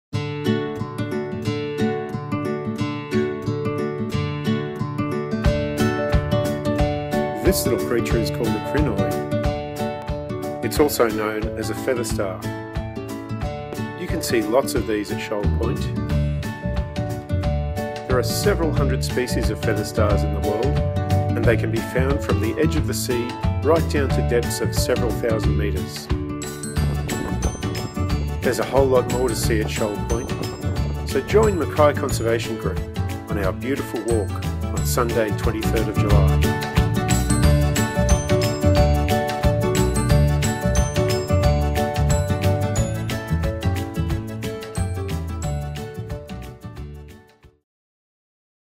Shoal Point Beautiful Walk 2017
Shoal Point Low Tide Beautiful Walk
23 July 2017, 3pm-5pm
Cnr Denman Ave & O'Brien Esplanade, Shoal Point

It has been a while since we held a Beautiful Walk so we decided to begin again with one of our favourites, at Shoal Point. We have timed this walk to coincide with a very low tide when all of sandy and rocky foreshore will be exposed.

You'll see lots of amazing creatures such as this feather star. Feather stars are crinoids and related to starfish and sea urchins. They have been described as "the most peaceful and beautiful starfish." It is very pleasing to watch their delicate tentacles wave in the tidal currents as they capture their food.

Shoal Point has an abundance of sea life. It's hard to know what will turn up with each tide but we have seen soft corals, spider crabs, starfish, sea urchins and many other creatures on previous visits. Bring your camera, you won't be disappointed.

We'll be walking in places where there are a few rocks and shallow water so please wear enclosed footwear. Feel free to bring some food and stay around for a chat after the walk.

As with all our Beautiful Walks, there is no charge for participating. Places are strictly limited.